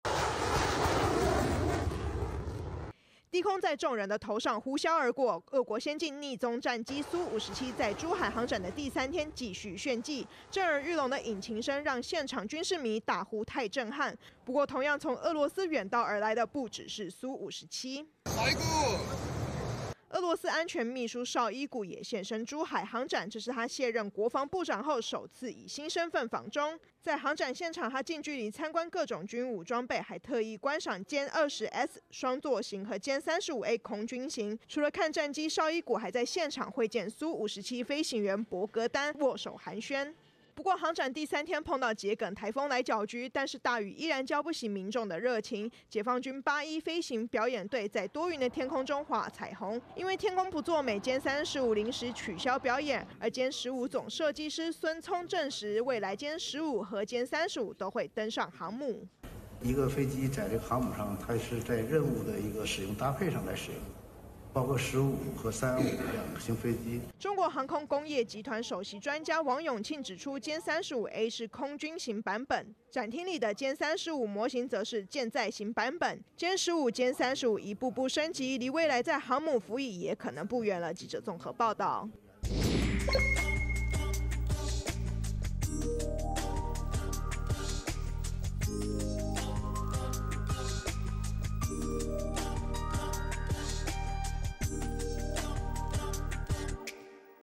【每日必看】"紹伊古"換新身分訪中 現身珠海航展武器館20241114
俄羅斯聯邦安全會議秘書"紹伊古"訪中的行程滿檔。參加完北京中俄戰略安全磋商後，今天(14日)現身珠海參觀中國航展，而儘管珠海天候不佳，包括俄羅斯隱形戰機，以及八一表演隊，依舊登場表演
責任編輯LUKE編